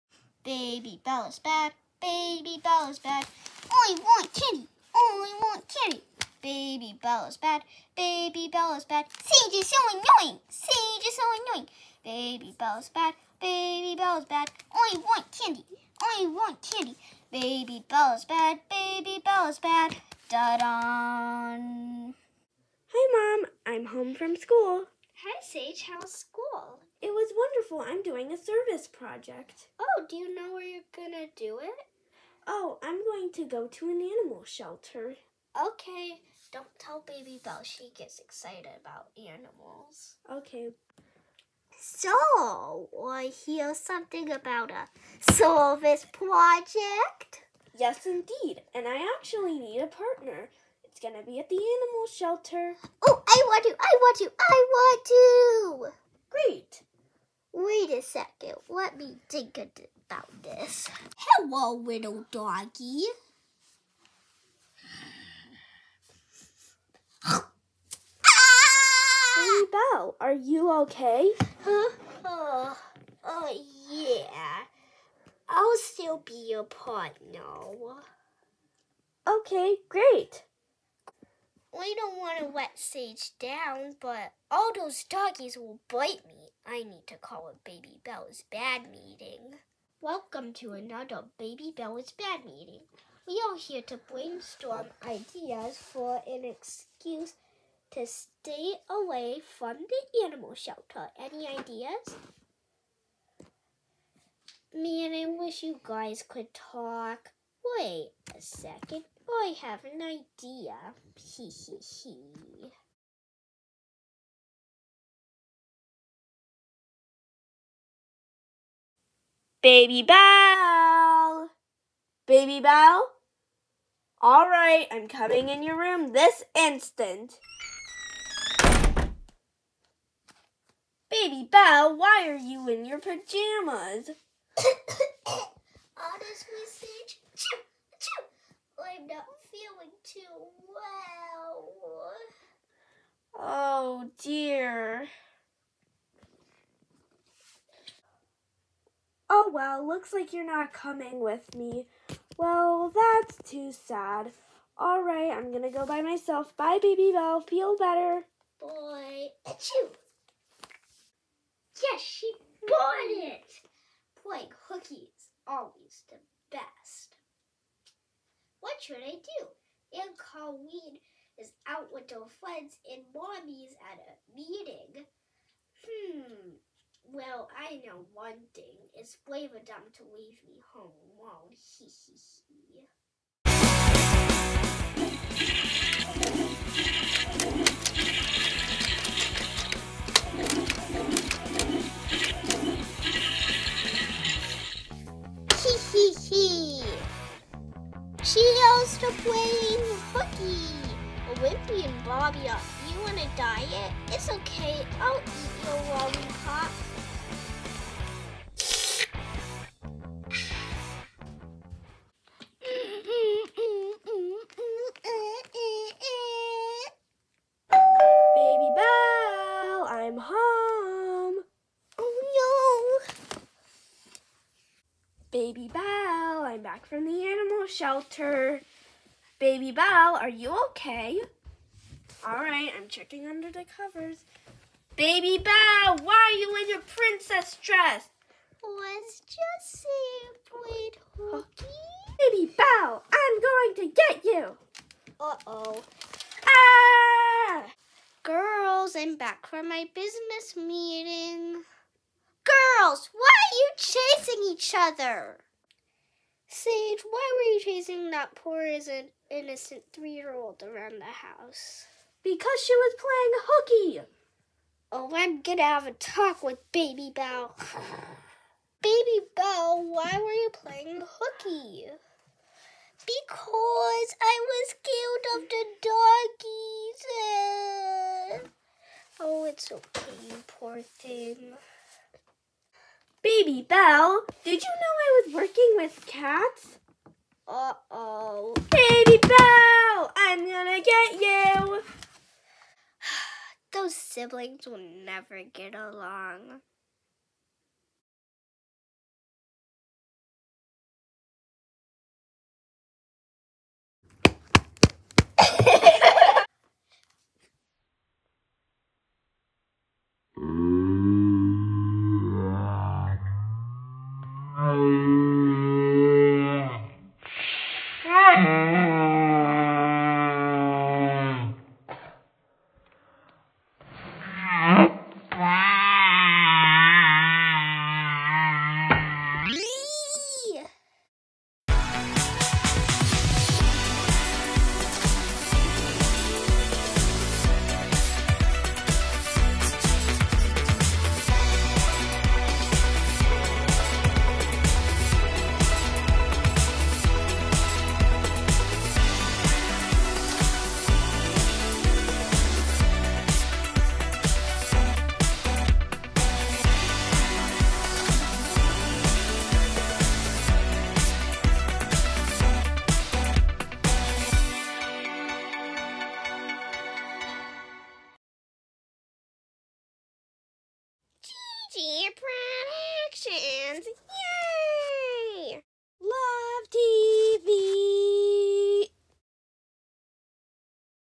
Baby Belle is bad S:1 Episode:3 Playing Hooky
Sage is doing a school project and Baby Belle doesn’t want to go. Baby Belle pretends to be sick.